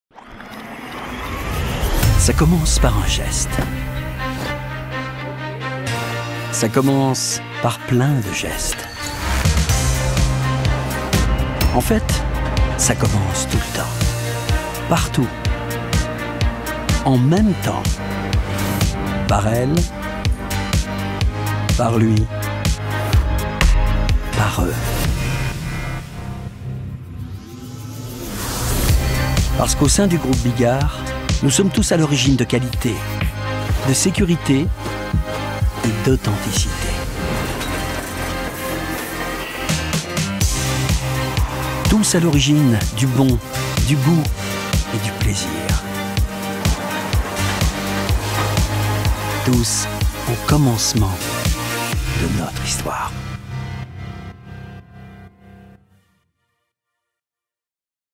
Groupe Bigard - Film corporate RH
Le groupe Bigard, c’est une histoire pas comme les autres. C’est une histoire avec une infinité de débuts et sans jamais de fin. Il y a eu le point de départ de 1968, puis plein de départs s’y sont agrégés : nouvelles décisions, nouvelles orientations, mais aussi chaque nouvelle personne recrutée, chacune apportant sa pierre à l’édifice, sa contribution à l’histoire du groupe.